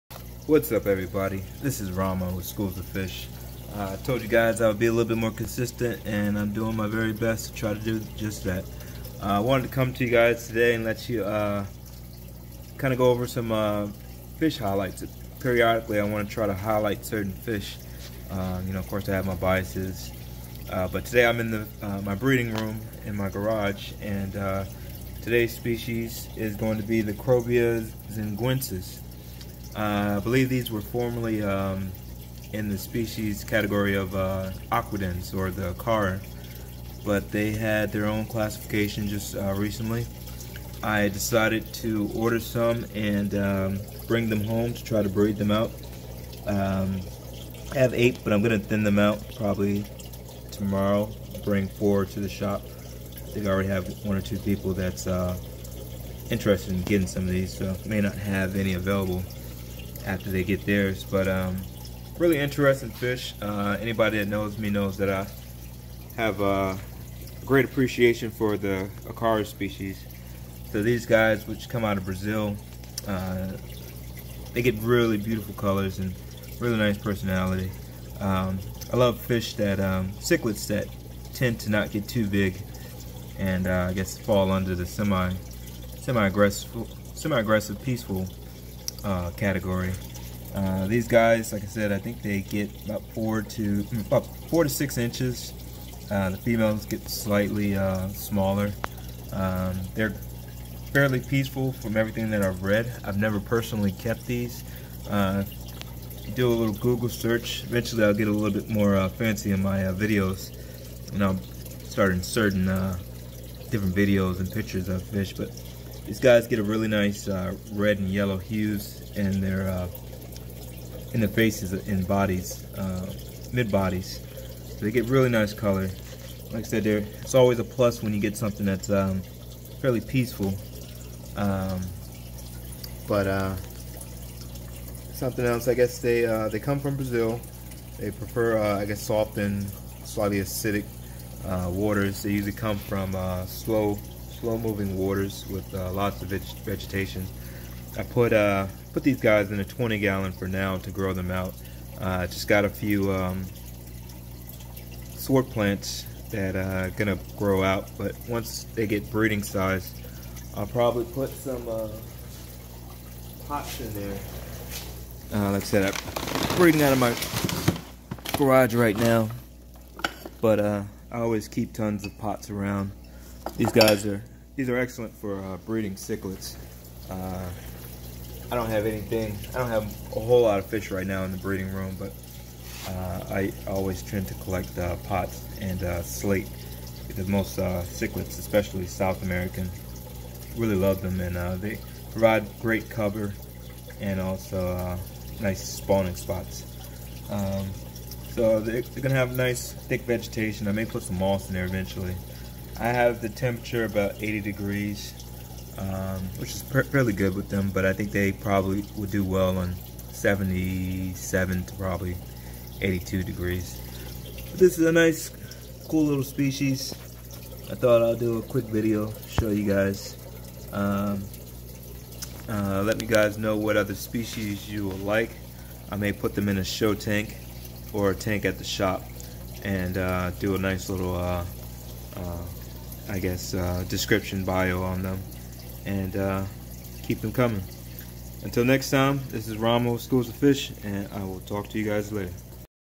Krobia Xinguensis-Species Profile
quick video highlighting Krobia Xinguensis that I'm growing out to breed.